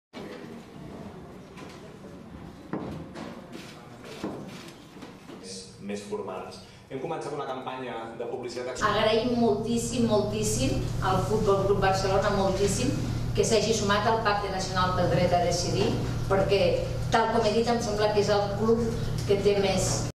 Spain: Catalan leader thanks FC Barcelona for supporting the "right to decide"
M/S Panel at 'Ara es L'hora' press conference

M/S Press

SOT, Carme Forcadell, ANC president (Catalan): "We thank FC Barcelona so
much for having joined the National Pact for the right to decide, because
its the club with the most followers in Catalonia right after social
security."

M/S Press

SOT, Carme Forcadell, ANC president (in Catalan): "Civil society is
conscious and we are conscious of our responsibility. That's why we have to
continue pushing political parties, Catalonia's government, and
institutions. We have to continue pushing and go all together, and that´s
what we want to show with the big event on October 19th."

W/S M/S Panel at 'Ara es L'hora' press conference and press

Muriel Casals, OMINUM CULTURAL president (English): "We found out that
those people [Blue State Digital] could be very useful for our campaign and
it turned out that these people had worked for Obama campaign and for the
Dilma Rouseff campaign with very good success so we are happy to have them."

M/S Exterior of National Assembly of Catalonia (ANC) building

SCRIPT:

The National Assembly of Catalonia (ANC) unveiled plans for their
self-determination campaign later this month during an 'Ara es L'hora'
conference in Barcelona, Friday.

The group plans to hold a large rally in support of the Catalan
independence movement on October 19, ahead of November’s referendum. During
the conference, the president of ANC, Carme Forcadell, also thanked FC
Barcelona for their decision to support Catalan self-determination.

The campaign will be run by Blue State Digital, the same company that
worked on the successful elections of Dilma Rouseff in Brazil, 2011, and
Barack Obama in 2009. The Catalan self-determination referendum is expected
to take place on November 9.